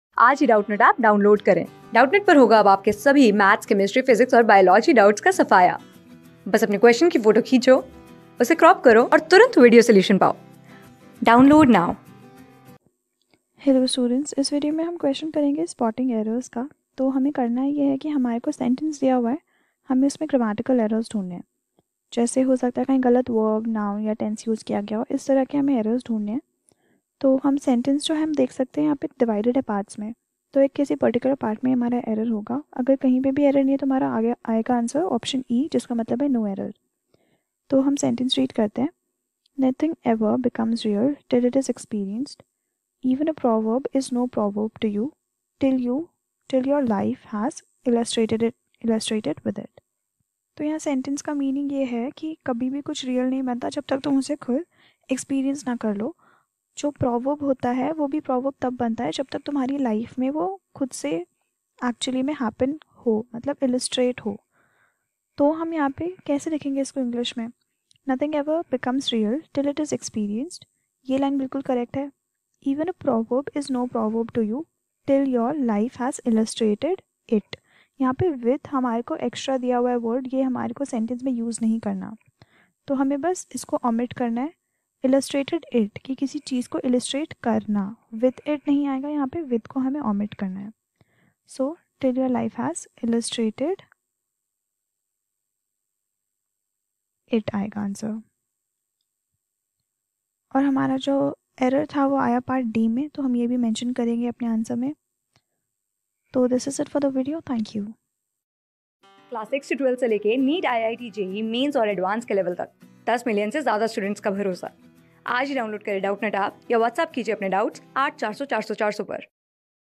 Read each sentence to find out whether there is any error in it. The error, if any, will be in o...
Read each sentence to find out whether there is any error in it. The error, if any, will be in one part of the sentence. The letter of that part is the answer, if there in no error, the answer is ( e ). (Ignore errors of punctuation, if any). Nothing ever becomes real (a)/ till it is experienced. (b)/ Even a proverb is no proverb to you (c)/ till your life has illustrated with it. (d)/ No error ( e )
Class: 14
Subject: ENGLISH
Chapter: SPOTTING ERRORS
Board:GOVT

👉 Have Any Query? Ask Us.